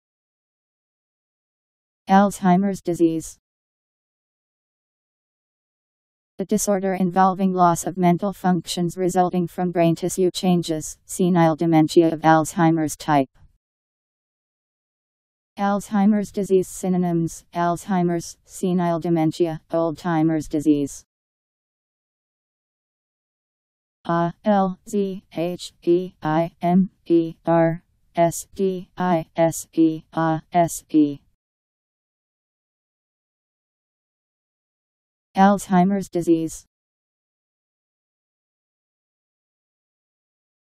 Alzheimer's disease Meaning
Video shows what Alzheimer's disease means. A disorder involving loss of mental functions resulting from brain tissue changes; senile dementia of Alzheimer's type.. Alzheimer's disease synonyms: Alzheimer's, senile dementia, old timer's disease.  Alzheimer's disease pronunciation. How to pronounce, definition by Wiktionary dictionary. Alzheimer's disease meaning. Powered by MaryTTS